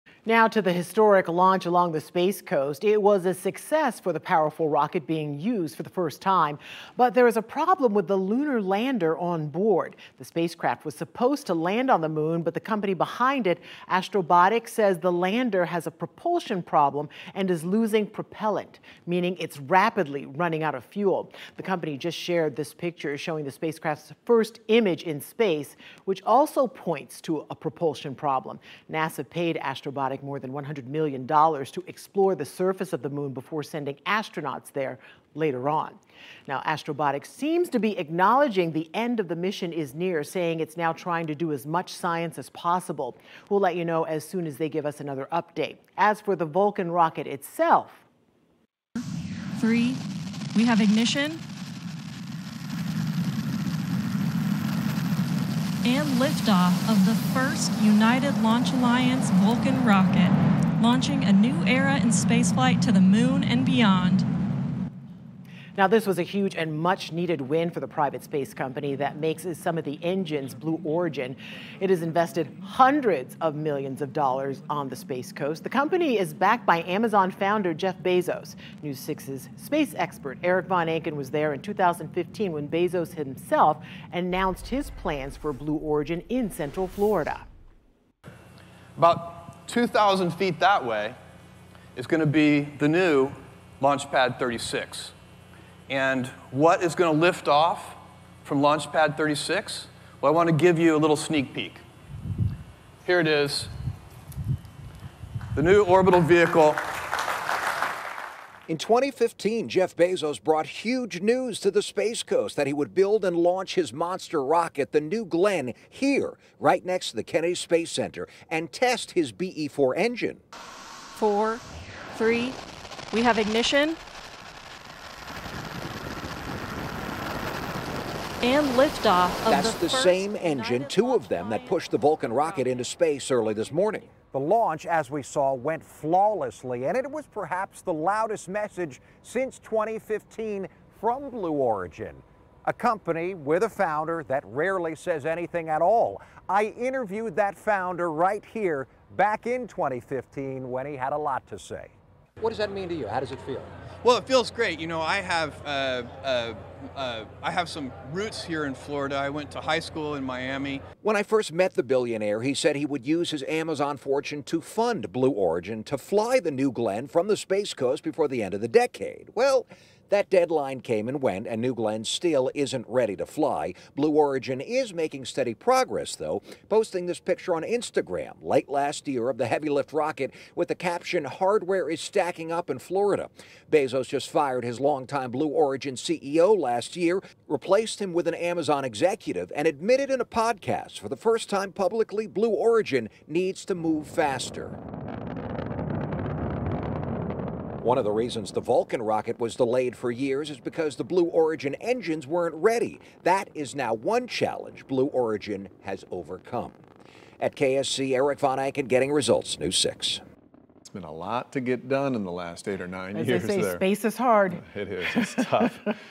ULA Vulcan Centaur rocket blasts off on inaugural launch; lunar lander develops problem on way t...
United Launch Alliance’s Vulcan Centaur rocket blasted off on its maiden launch early Monday from Florida’s Space Coast.